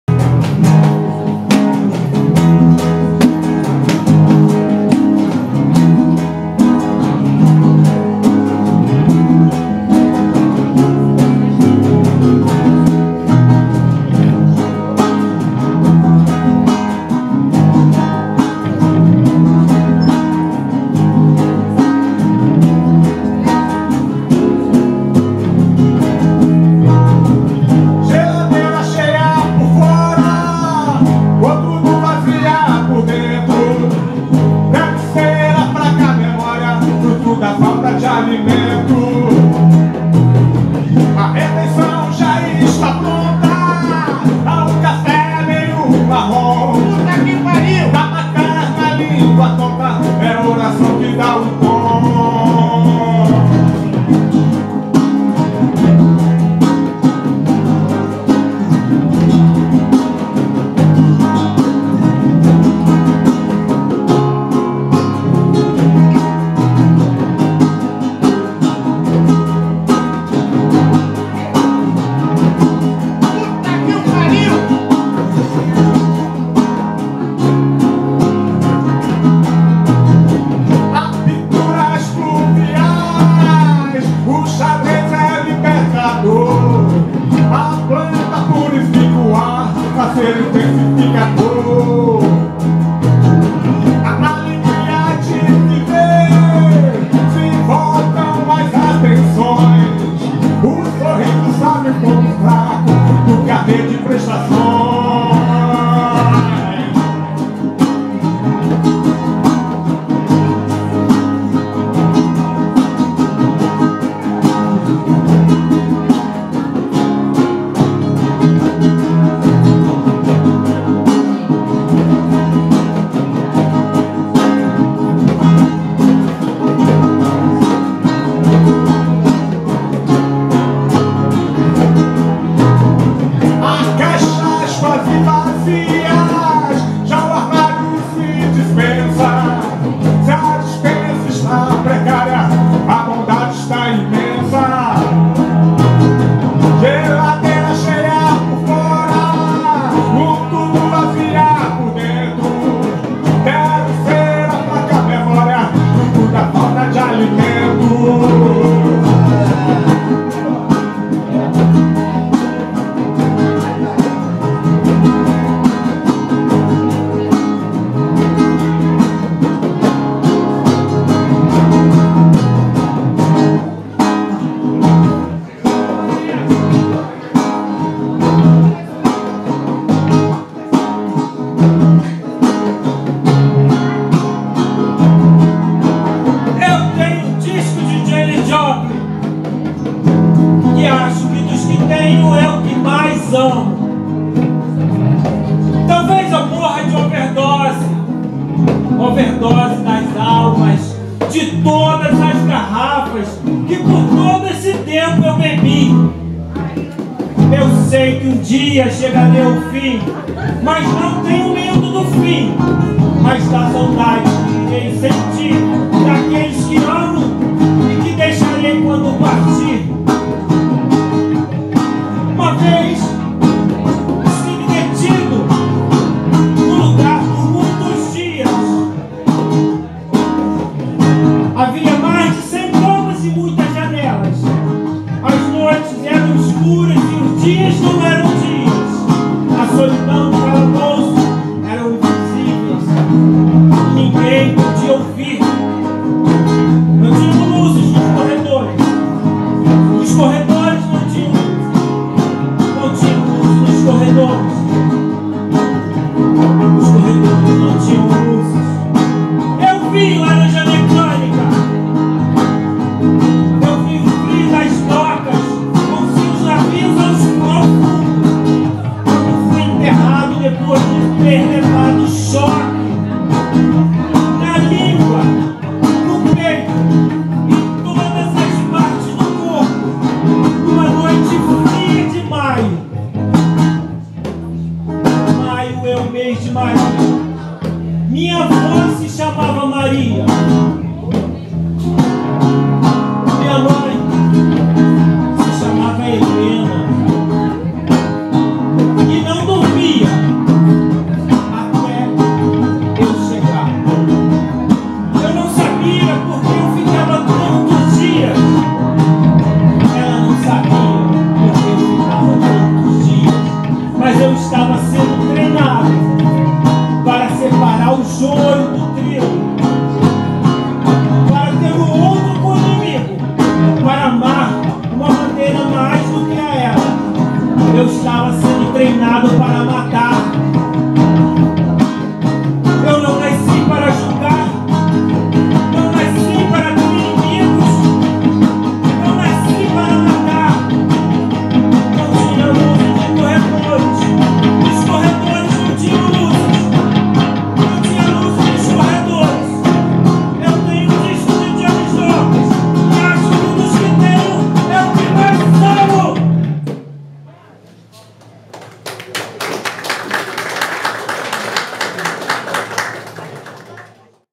Banquete para Esquecer / Overdose (Live Encontro de Poetas & Afins)
Lançamento da 16ª edição do Gambiarra Profana no SARAU POETAS & AFINS (ano VIII/ ed. 66), que reúne poetas da Baixada Fluminense e do município do Rio de Janeiro, na edição do dia 31/05/18.

Neste vídeo é apresentado dois textos da edição 16: "Banquete para Esquecer (A melancolia Escondida dentro de um Pão com Manteiga)" de Jonatan Magella e "Overdose" de Arnoldo Pimentel ambos executados por Fabiano Soares da Silva, Sergio-SalleS-oigerS e Vagner Vieira.